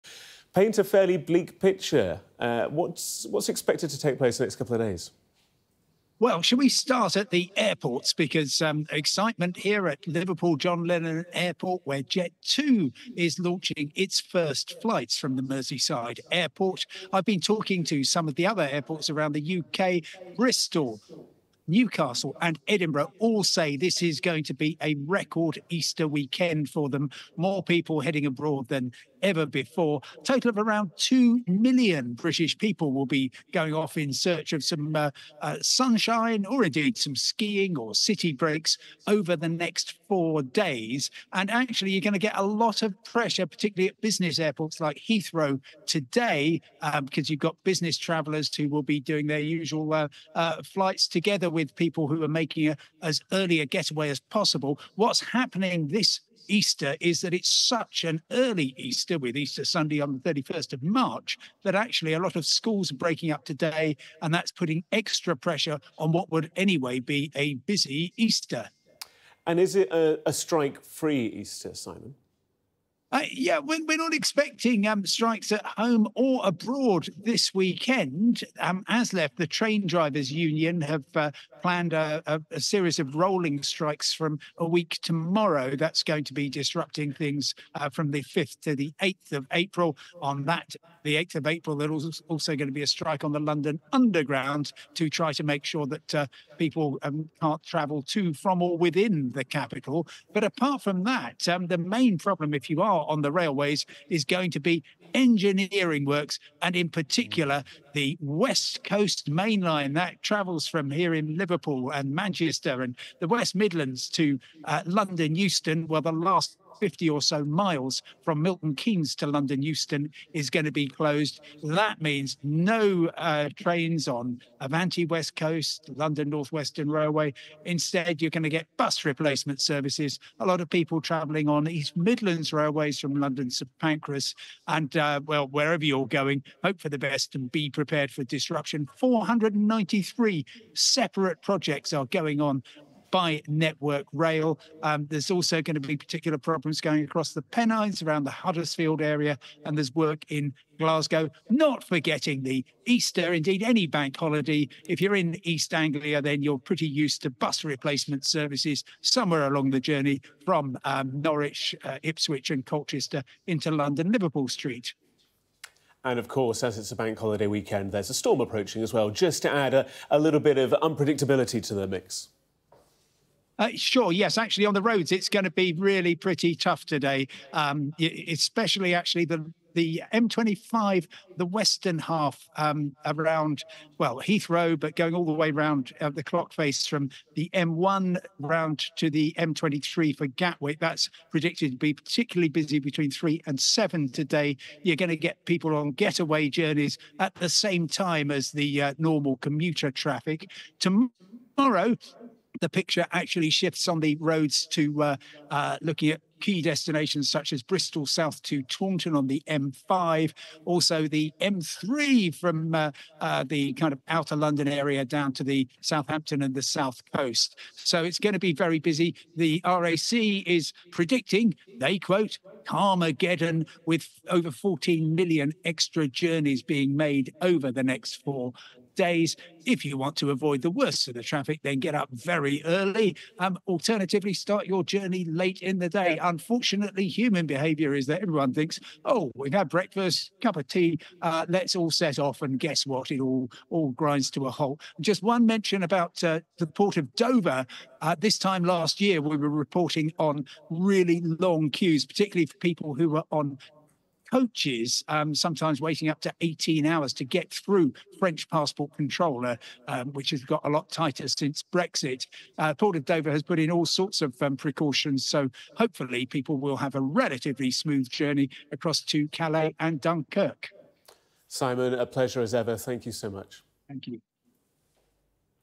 Easter weekend: Disruption expected for travellers
Easter weekend is almost here and drivers are being warned of "carmaggedon" as millions of people take to the roads.

Those making getaways could also find themselves caught in delays by ferry, road and air.

The Independent's travel correspondent, Simon Calder spoke to us about the weekend's travel.